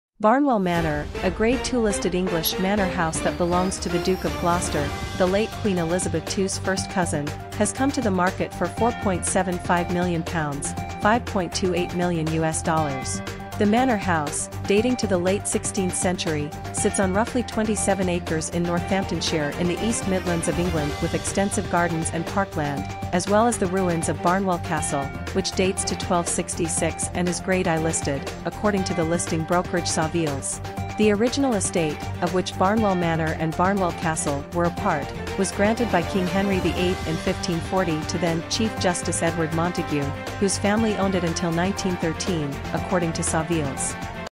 Historic Manor House: Barnwell Manor is for Sale £4.75 Million
Description: Barnwell Manor, a Grade II-listed English manor house that belongs to The Duke of Gloucester, the late Queen Elizabeth II’s first cousin, has come to the market for £4.75 million (US$5.28 million).